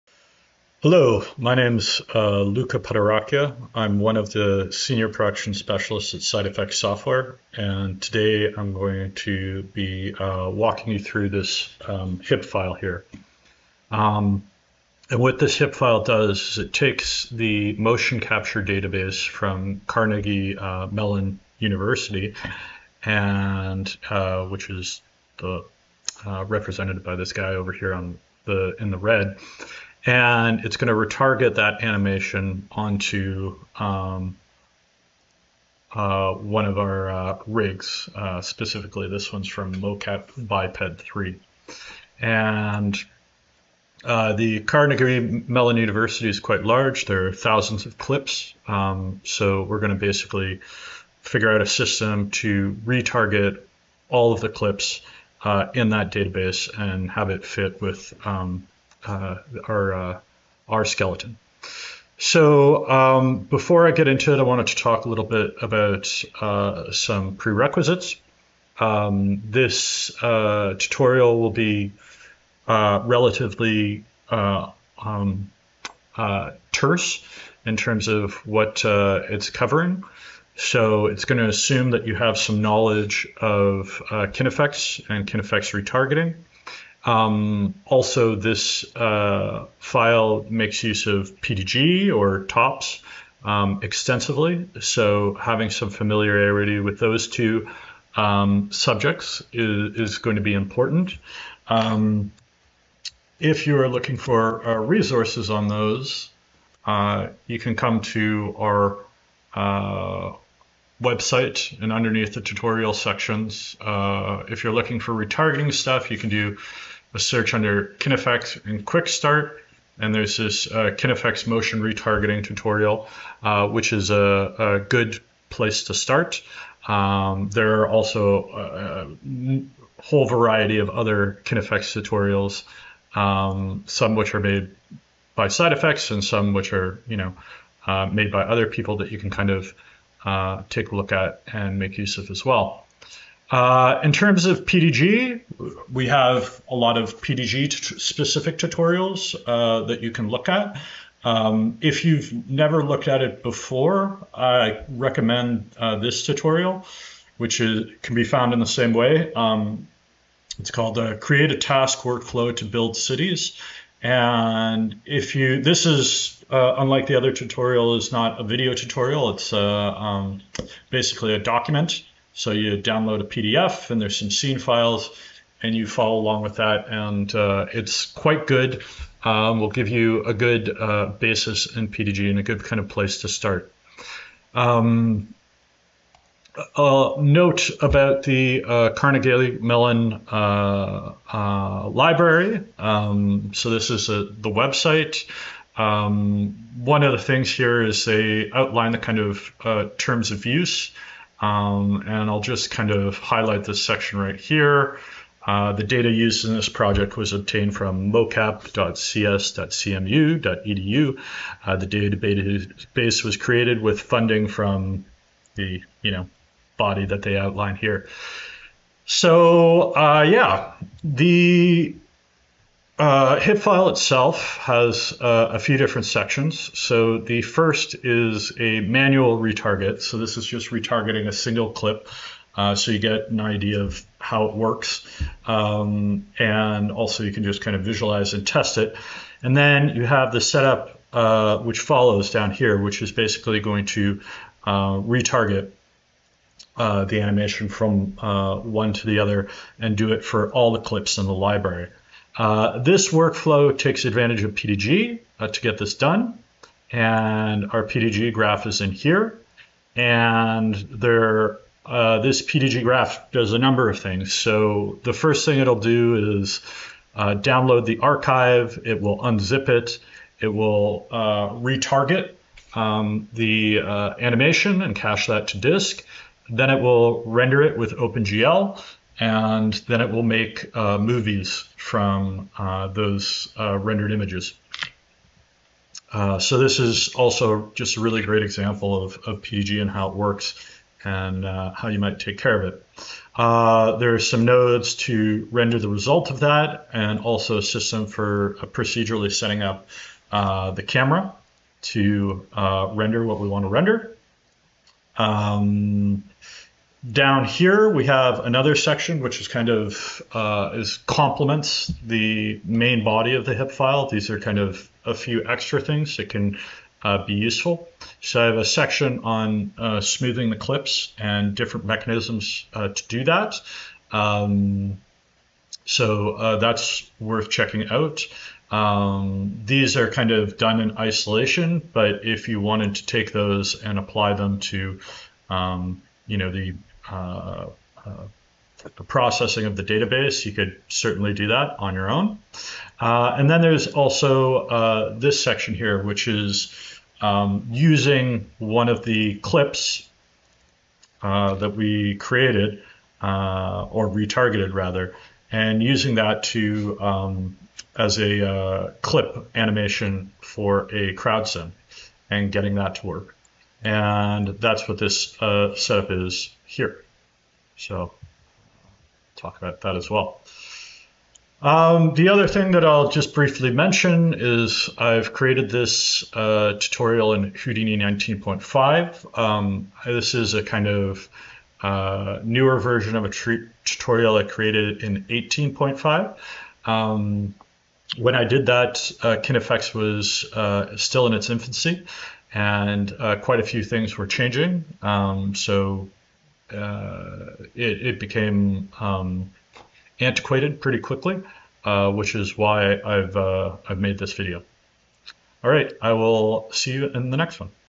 Retarget the CMU Database 1 | Introduction and Prerequisites
Learn what is going to happen in this lesson and required prerequisites and resources.
There will also be a quick overview of the scene file.